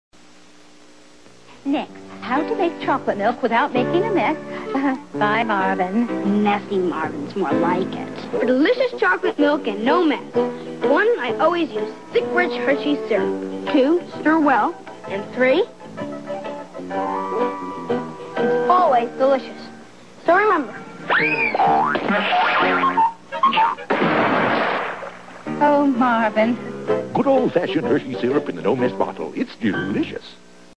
Hershey Syrup Messy Marvin at School Commercial (1982)
A great ad in the "Messy Marvin" lineup of commercials starring Peter Billingsley, who famously played Ralphie from "A Christmas Story."  These commercial were a staple on TV for years, and it was almost a rite of passage for a kid to connect the young actor to the famous Christmas movie he starred in.